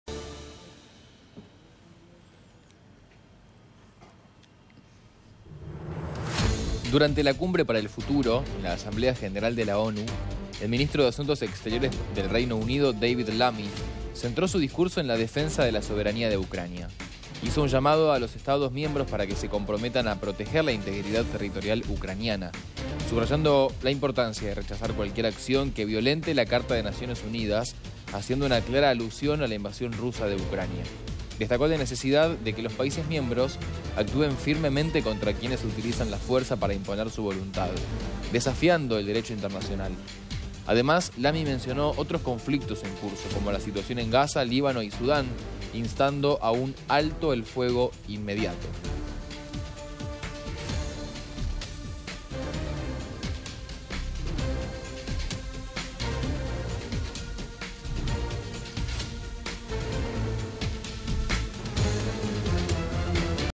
Reino Unido defendió la integridad territorial de Ucrania en la ONU
👉 Durante la "Cumbre para el Futuro" en la Asamblea General de la ONU, el ministro de Asuntos Exteriores del Reino Unido, David Lammy, centró su discurso en la defensa de la soberanía de Ucrania.